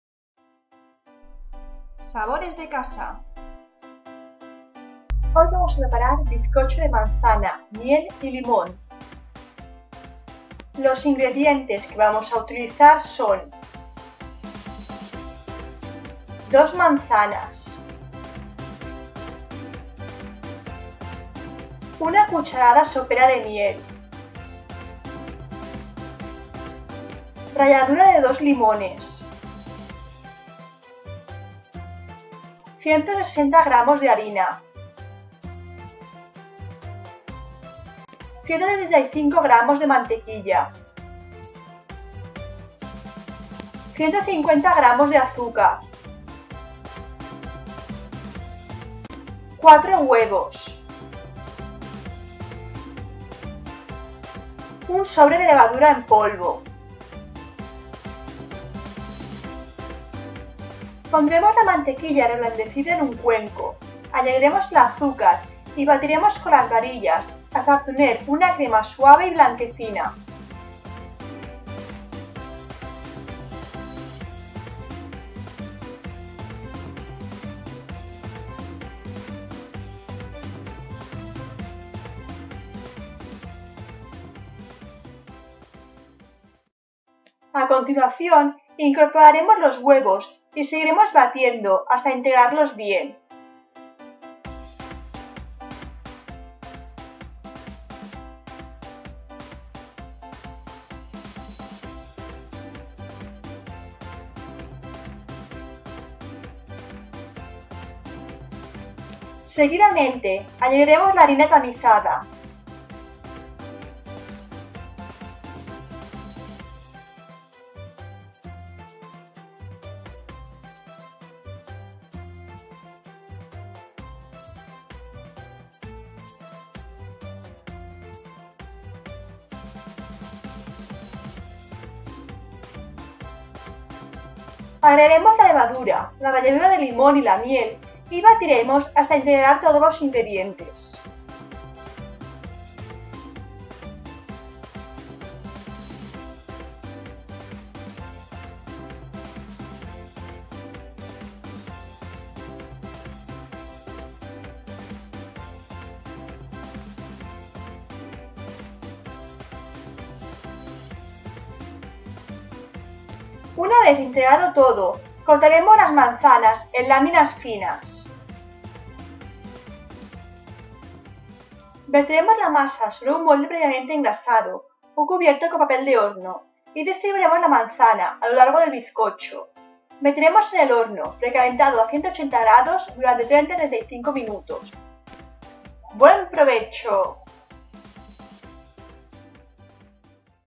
Bizcocho de manzana, miel y limón
Bizcocho de manzana, miel y limón, ideal para cualquier ocasión. Ingredientes principales: manzana, miel, limón, harina , huevos. Tiempo de preparación: 50min.